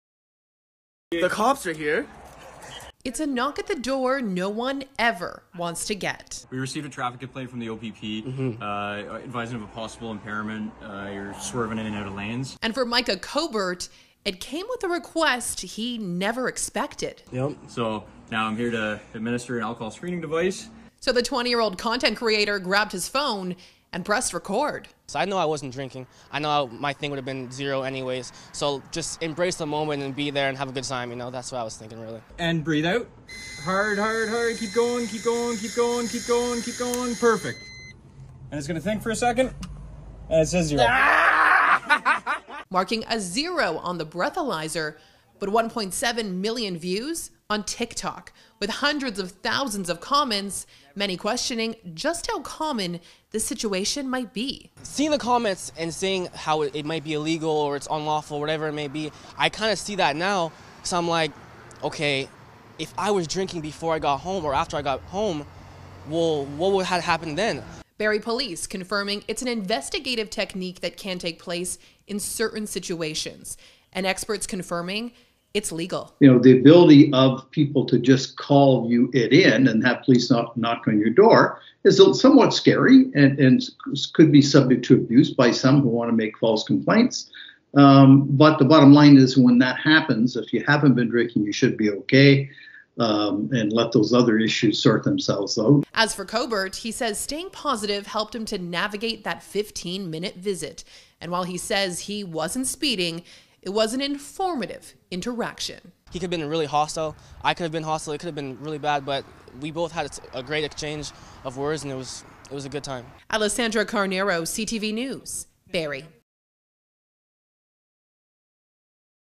Police demand breathalyzer test at Ontario man's home
Police spark a social media debate after showing up at man's door for a breathalyzer test based on traffic complaint.